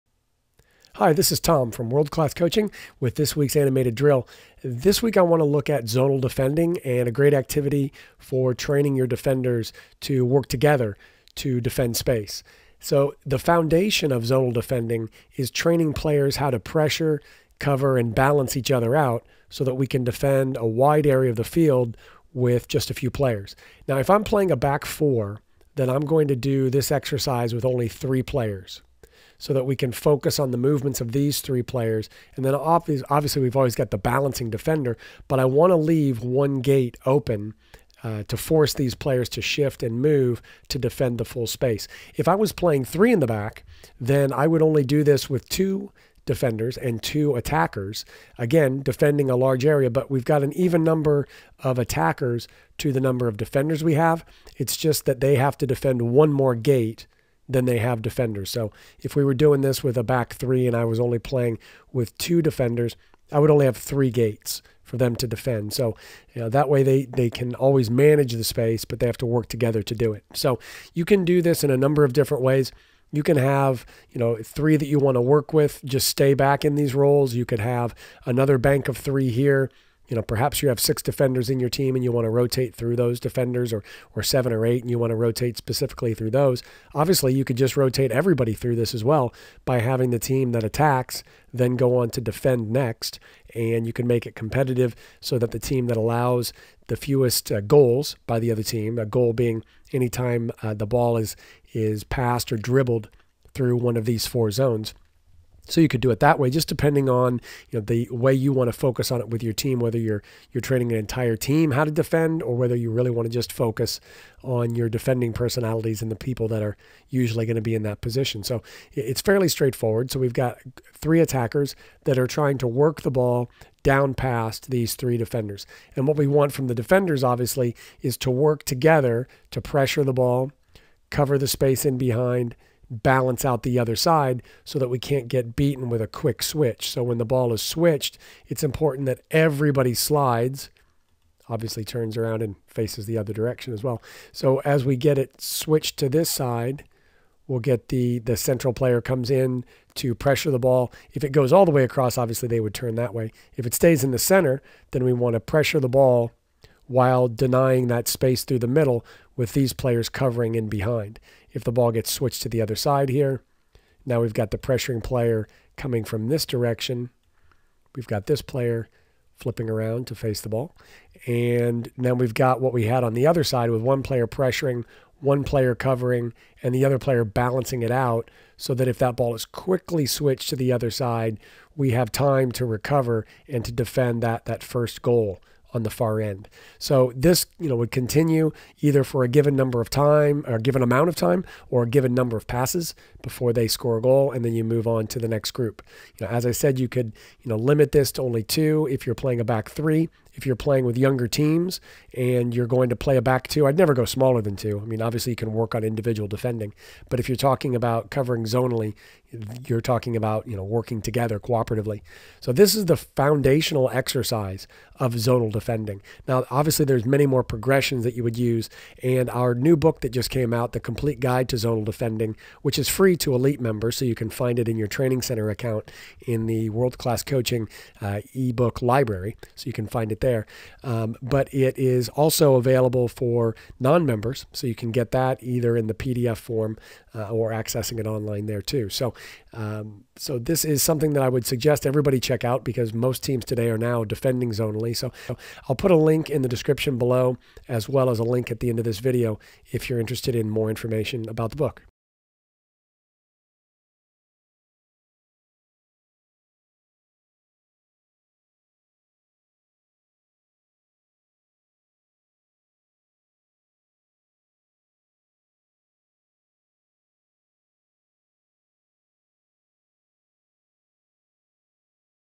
Training Zonal Defending
The Complete Guide to Zonal Defending

This is a foundational exercise for training your team to defend zonally. The activity can be progressed to include the whole team in order to teach them to defend collectively.